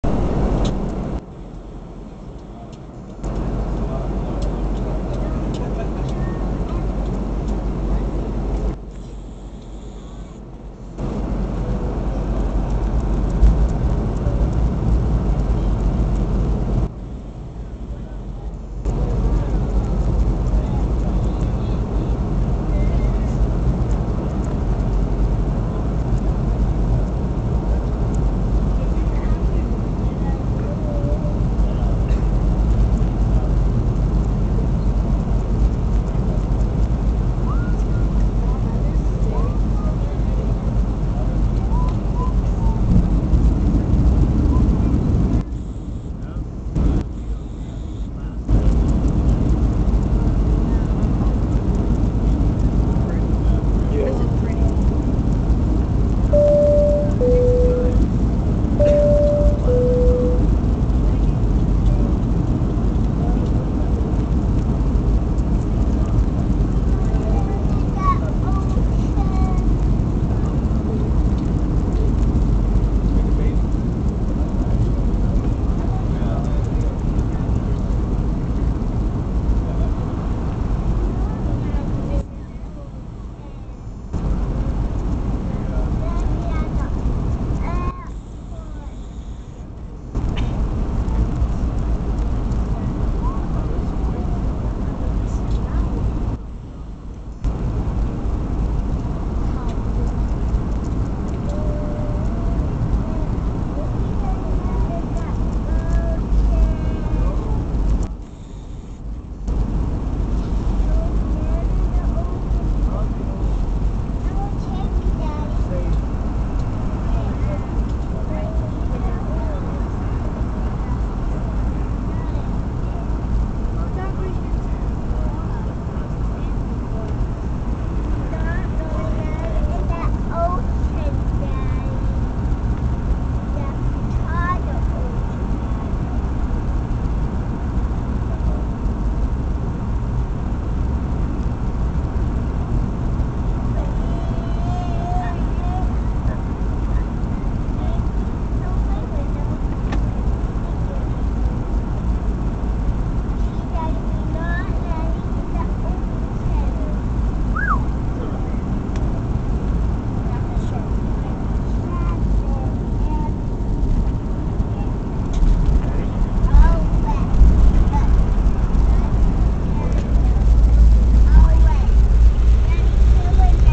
airplane landing in hawaii
our landing in honolulu hawaii after a long ass fligh